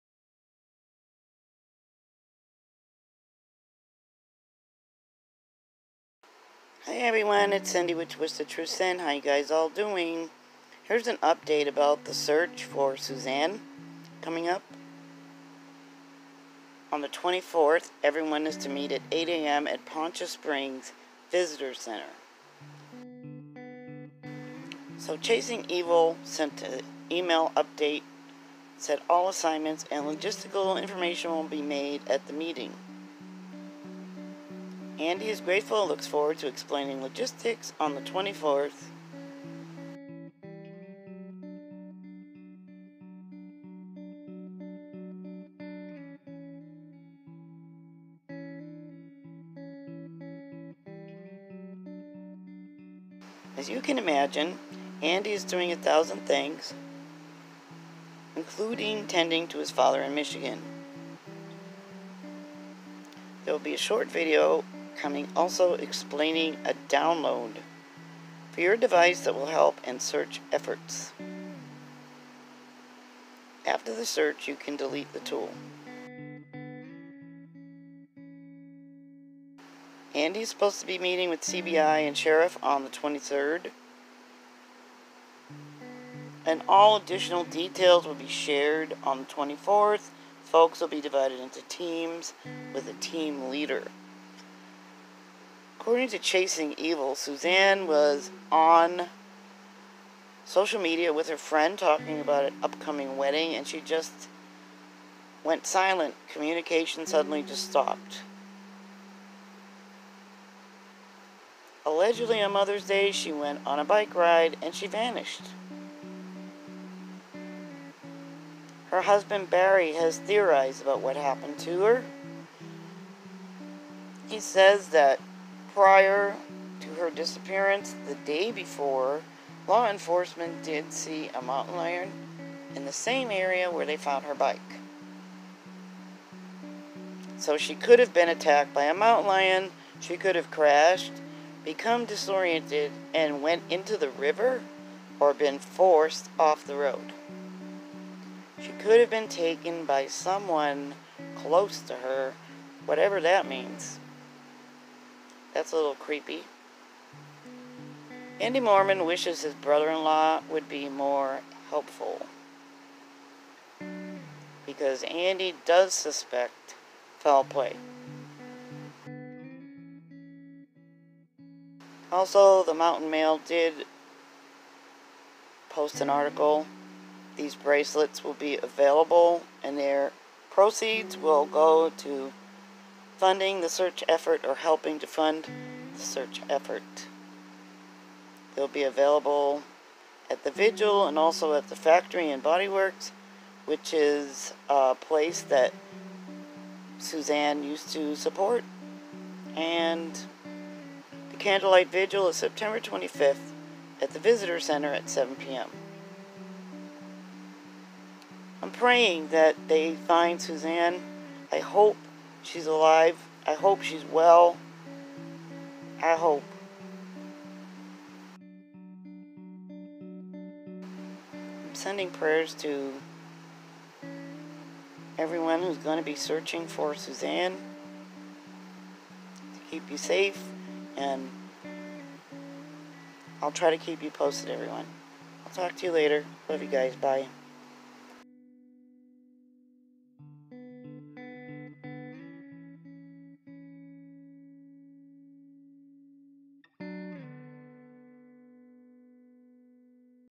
Suzanne Morphew Search Update from Profiling Evil
The search for Suzanne begins at 8 am on Sept 24, 2020 at Poncha Springs Visitor Center in Colorado. Assignments will be given the morning of Sept 24. 2020.

Suzanne Morphew's brother Andy Moorman will meet with CBI and Chaffee County Sheriff on Sept 23, 2020.

Prayer Vigil will be on Friday, Sept 25 at 7 pm - Riverside Park in Salida, Co.